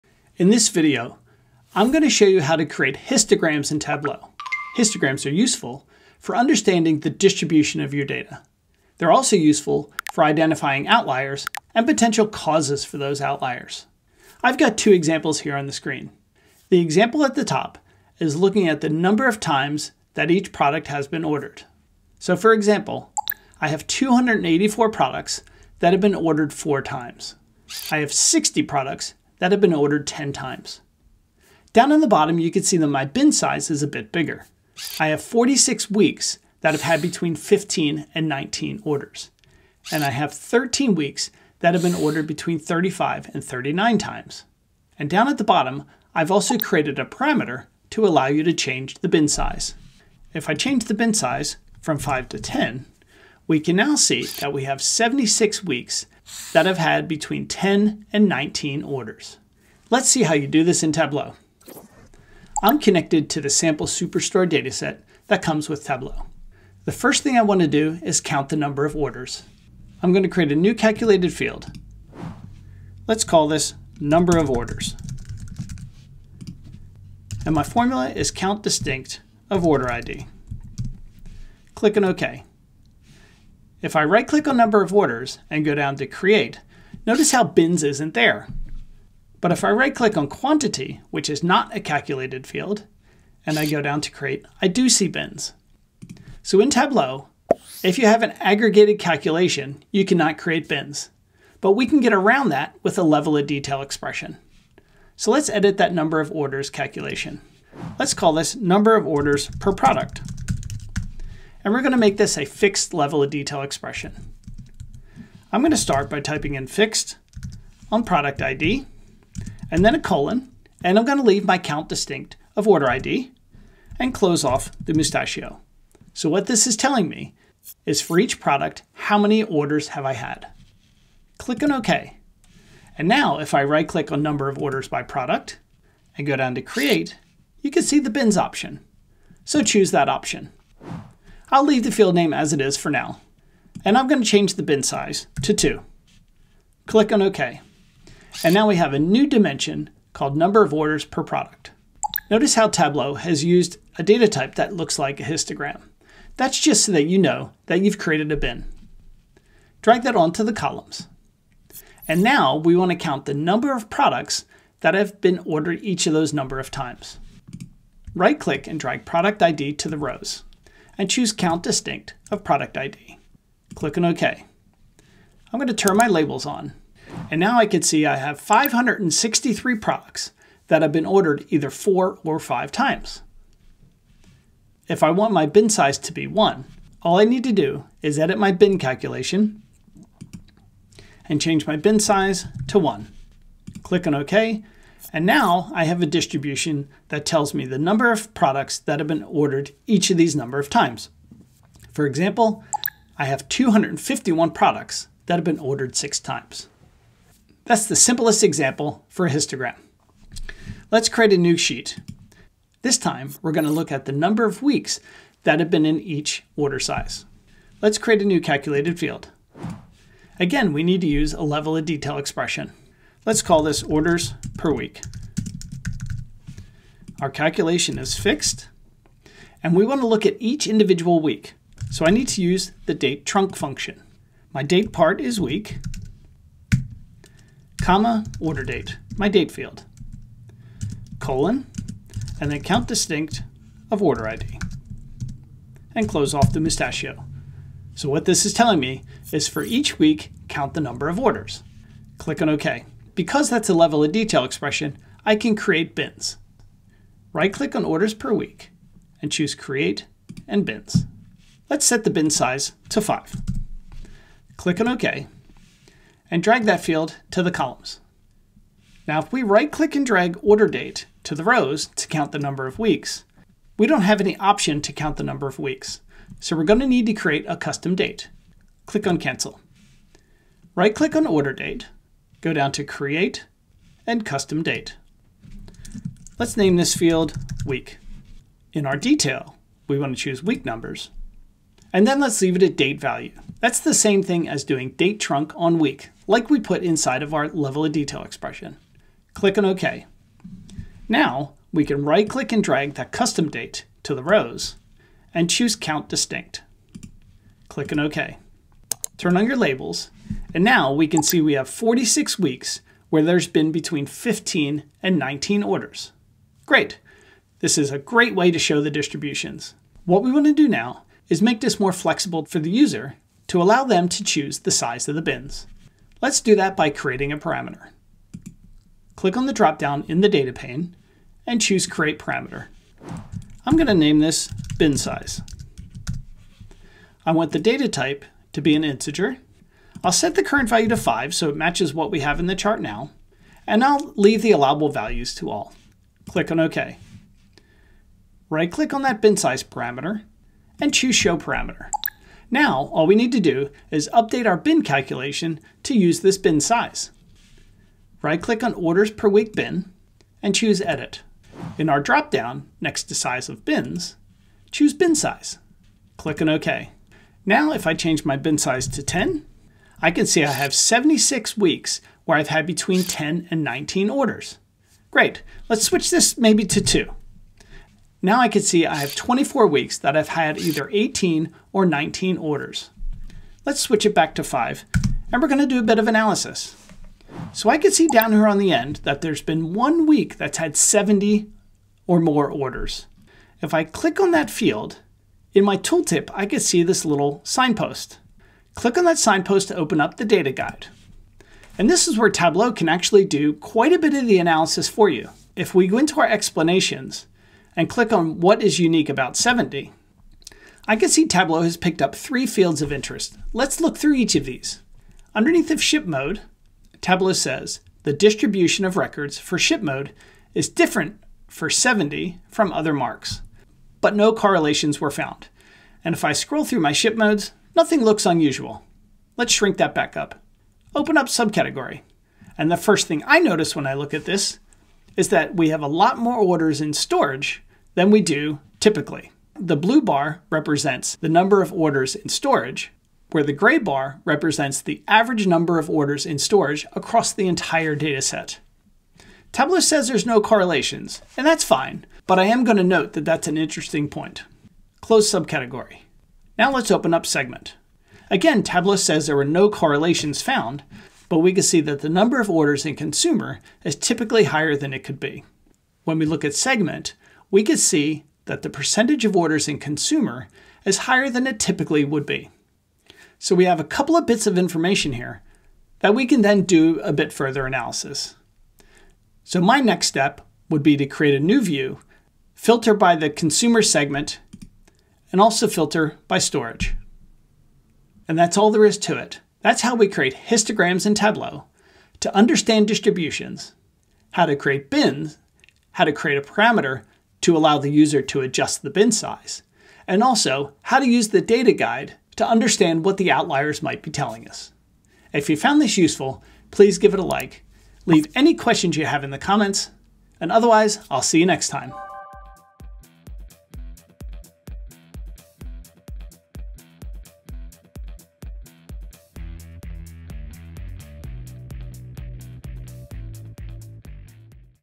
Harnessing Histograms for Data Analysis in Tableau
Explore the full spectrum of data visualization with Tableau histograms and the Data Guide feature!

This comprehensive video tutorial takes you on an exploratory journey from the basics of histograms to the advanced insights offered by Tableau's Data Guide.

In this instructional video, you will:

- Grasp the concept of histograms and their role in revealing data distributions.
- Tailor bin sizes and intervals in Tableau for customized data analysis.
- Utilize Tableau's Data Guide to enhance your understanding and interpretation of histograms.
- Apply your knowledge for practical learning.

Additionally, I'll walk you through the Data Guide in Tableau - a powerful feature that aids in decoding your data's story, ensuring you're not just seeing data but truly comprehending it.

By the end of this tutorial, you'll not only master the creation of histograms in Tableau but also leverage the full potential of the Data Guide to make informed decisions.

NEWSLETTER
Every week I provide advice, tips, live stream videos, discounts on products I create, and much more.